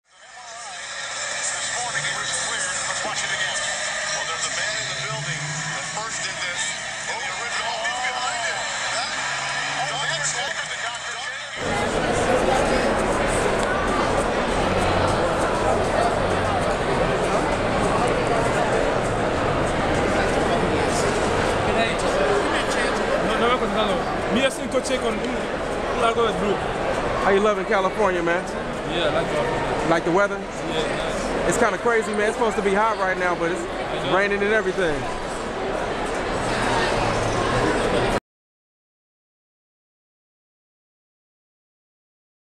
Serge Ibaka After Dunk From Free Throw Line
Oklahoma's own Serge Ibaka killedm on that free throw line . jordan ant got nun on that one . Full Fledge is everywere your not we got it on lock .. check out this exclusive interview